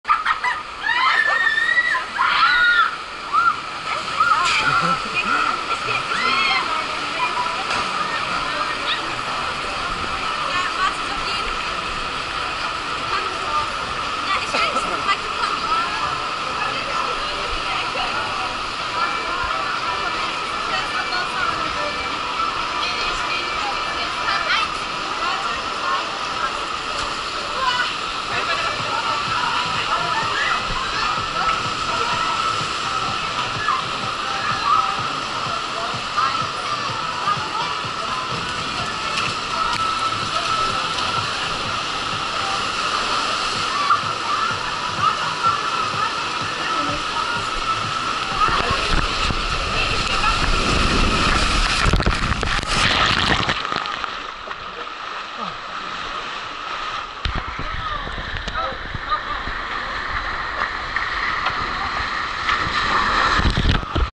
Breitrutsche Water Slide at Area 47
Breitrutsche water park slide at Area 47 aqua park in Ötztal, Tyrol, Austria.